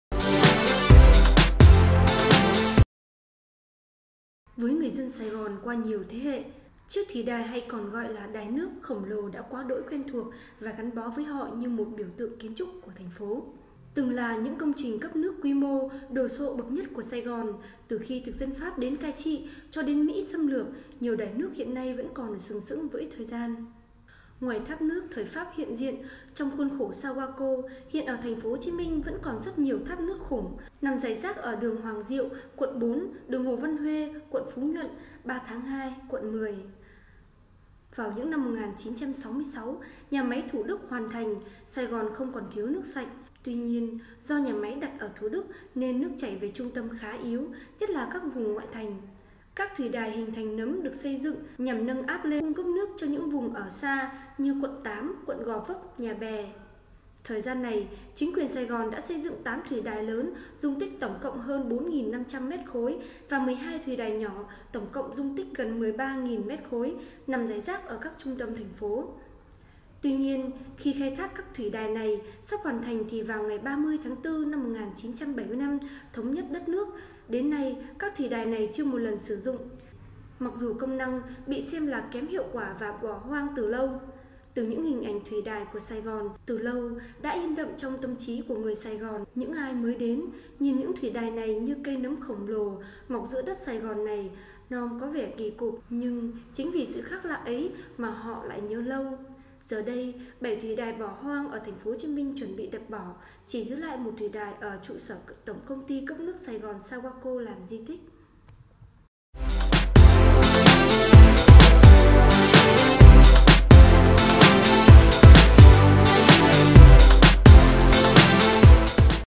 Hình ảnh khắc sâu trong tâm trí mỗi người Sài Gòn
Với người dân Sài Gòn qua nhiều thế hệ, chiếc thủy đài (đài nước) khổng lồ đã quá đỗi quen thuộc và gắn bó với họ như một biểu tượng kiến trúc của TP.
Từng là những công trình cấp nước quy mô, đồ sộ bậc nhất Sài Gòn từ khi thực dân Pháp đến cai trị cho đến Mỹ xâm lược, nhiều đài nước hiện nay vẫn còn sừng sững với thời gian.
Ngoài tháp nước thời Pháp hiện diện trong khuôn viên Sawaco, hiện ở TP.HCM vẫn còn rất nhiều tháp nước “khủng” được xây dựng trước năm 1975, nằm rải rác ở đường Hoàng Diệu (Q.4), Hồ Văn Huê (Q.PN), Ba Tháng Hai (Q.10)...
Vào năm 1966, Nhà máy nước Thủ Đức hoàn thành, Sài Gòn không còn thiếu nước sạch. Tuy nhiên, do nhà máy đặt ở Thủ Đức nên nước chảy về trung tâm khá yếu, nhất là vùng ngoại thành. Các thủy đài hình nấm được xây dựng nhằm nâng áp lên, cung cấp nước cho những vùng ở xa như Q.8, Gò Vấp, Nhà Bè...
Thời gian này, chính quyền Sài Gòn đã xây dựng 8 thủy đài lớn có dung tích tổng cộng hơn 48.500 m3 và 12 thủy đài nhỏ tổng cộng dung tích gần 13.000 m3 nằm rải rác ở trung tâm thành phố.
Tuy nhiên, khi các thủy đài này sắp hoàn thành thì vào 30/4/1975 thống nhất đất nước. Đến nay các thủy đài chưa một lần sử dụng.
Mặc dù công năng bị xem là kém hiệu quả và bị bỏ hoang đã lâu, nhưng hình ảnh những thủy đài Sài Gòn từ lâu đã in đậm trong tâm trí người Sài Gòn.
Những ai mới đến, nhìn những thủy đài như cây nấm khổng lồ “mọc” giữa đất Sài Gòn này nom có vẻ... kỳ cục. Nhưng chính vì sự khác lạ ấy mà họ lại nhớ lâu!
Giờ đây, bảy thủy đài bỏ hoang ở TP.HCM chuẩn bị đập bỏ, chỉ giữ lại một thủy đài ở trụ sở Tổng công ty Cấp nước Sài Gòn (Sawaco) làm di tích.